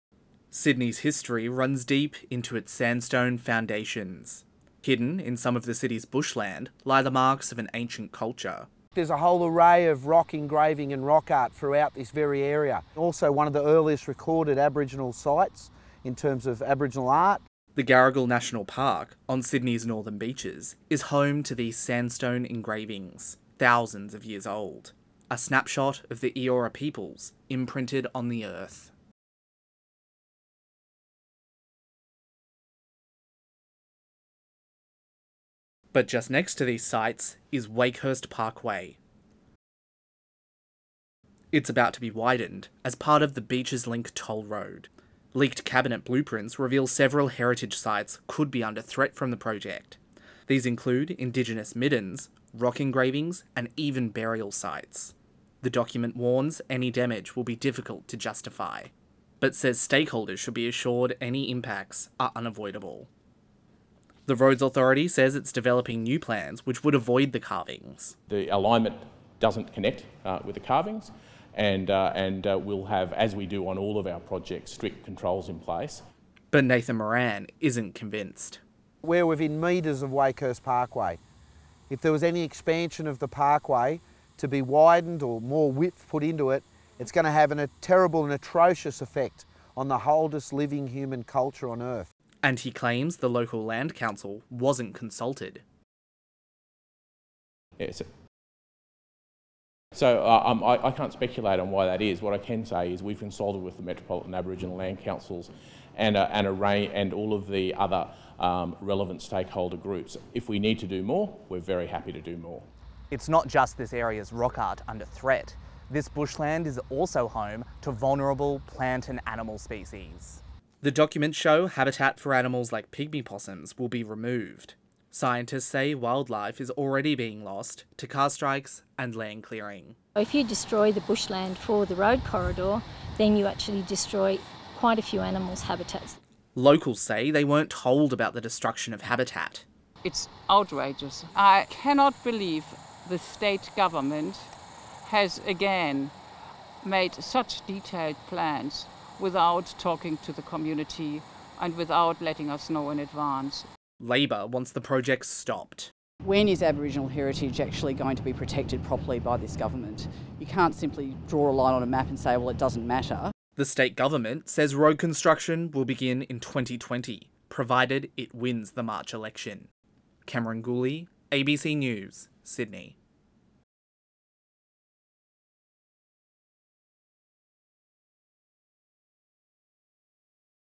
Sydney Indigenous Rock Art under pressure from New Freeway on Northern Beaches
Community groups say they want more consultation on a large new freeway project on the edge of one of Sydney's pristine national parks. Leaked documents reveal the richness of indigenous rock-art, thousands of years old on the edge of the project.